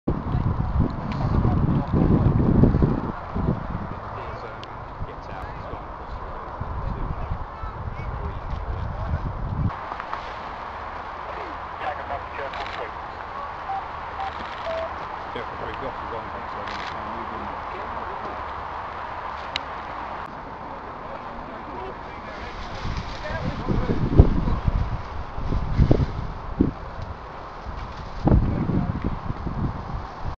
Horizon News: Loose Deer
Deer on the loose in Milton Keynes on 3/10/2008